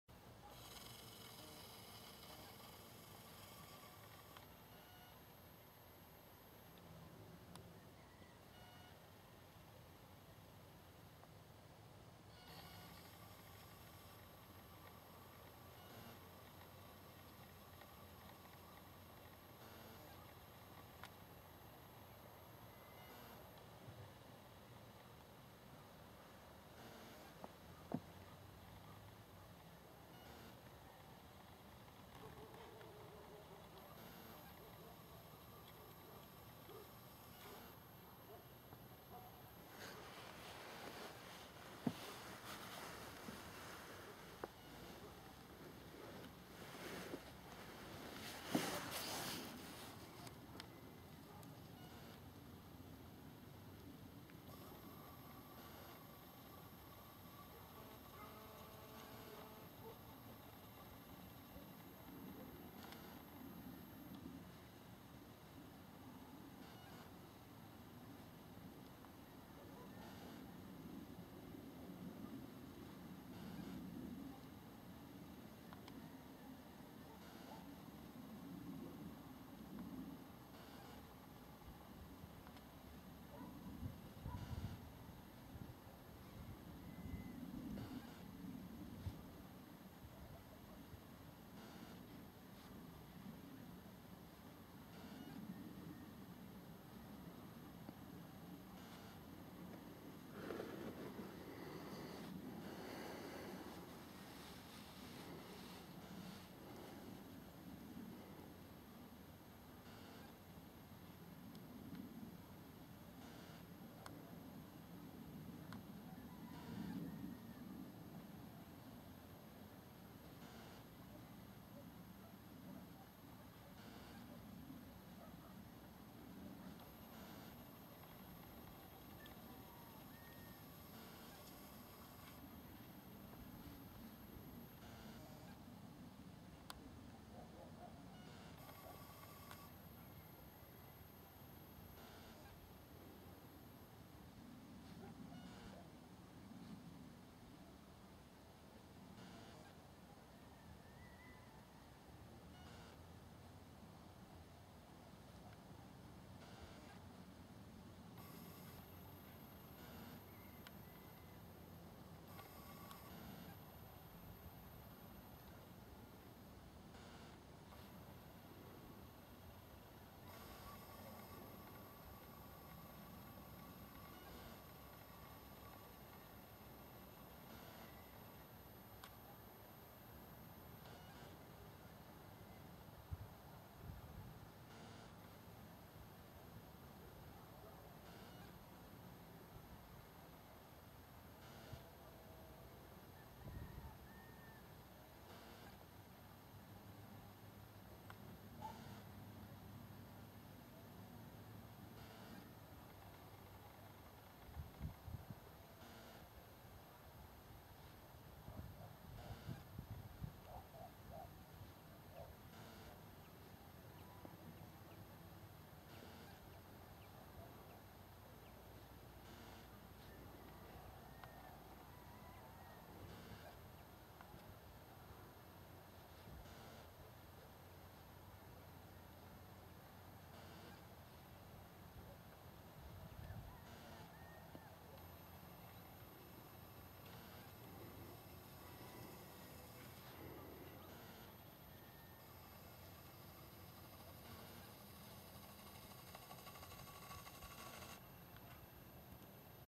:)) Y Y :))
Csavargás régi utakon. :) Érdemes volt. :) Sokáig kerestem ezeket a kedves állatokat és nem találtam. Most csak be akartam járni régi földutakat és megláttam ezeket a kedves...Őzeket?